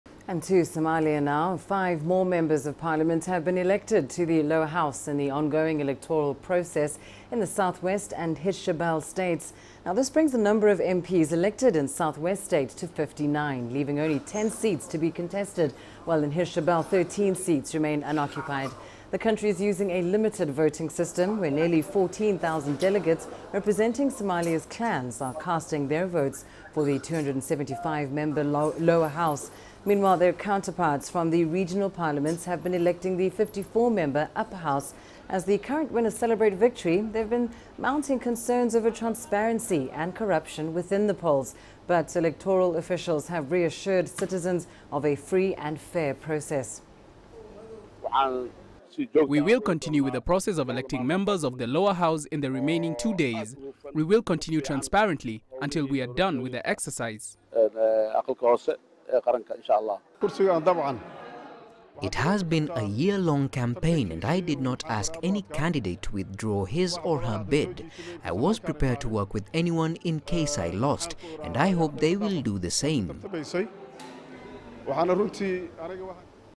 Five more members elected to Somalia's Lower House
Five more Members of Parliament have been elected to the Lower House in the ongoing Somalia electoral process, in the South West and Hirshabelle States. This brings the number of MPs elected in South West State to 59, leaving only 10 seats to be contested, while in Hirshabelle 13 seats remain unoccupied. The country is using a limited voting system, where nearly 14,000 delegates representing Somalia's clans are casting their votes for the 275-member Lower House. Meanwhile, their counterparts from the regional parliaments have been electing the 54-member Upper House. As the current winners celebrate victory, there have been mounting concerns over transparency and corruption within the polls. But electoral officials have reassured citizens of a free and fair process.